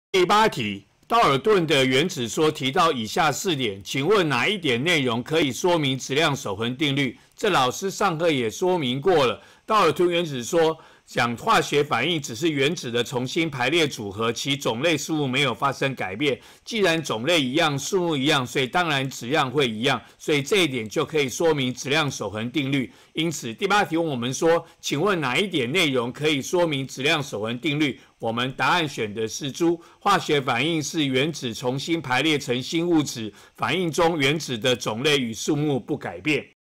109學年下8年級理化一段第08題
問道耳頓原子說的四點，哪一點內容可以說明質量守恆定律?